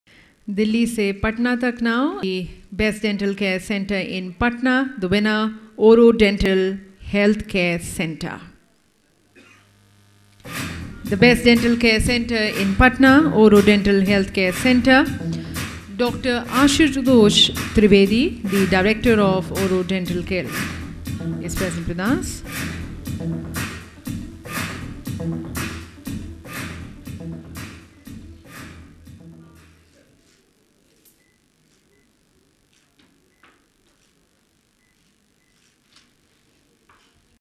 Oro Dental Health Care Centre got recognized at the PrimeTime National Dental Excellence Awards,2014
Dr. Trivedi, Oro Dental Health Care Centre got recognized at the Prime Time National Dental Excellence Awards, 2014

"National Dental Excellence Awards are to recognize significant contribution in the Dentistry sector. These are a small token of appreciation from Prime Time to honor the Achievers, Innovators and Leaders in a spectacular style. At the same time is conglomeration of the elites who join in to celebrate and felicitate excellence, reputation and exemplary work in Dentistry.

This National Dental Excellence Awards highlight, recognize and reward the participants’ ability to steer their businesses and services through turbulent times, applying the best of business modules to manage and keep their missions afloat.

The recipients of the awards are visionaries behind today's outstanding Dentistry Services. They are stellar individuals and organizations who epitomize strength, ingenuity, knowledge, foresight and who drive the growth of their businesses and services with vision and inspiration.

Graced by the presence of Dr. Anil Jain, Dr. O. P. Kharbanda, Dr. R. K. Bali were the Chief Guests for the event , Prime Time National Dental Excellence Awards, 2014 took place in Holiday Inn in Mayur Vihar, New Delhi on 11 August, 2013 & was covered by leading print and electronic media houses.

Fill up the nominations to Participate in the Upcoming Program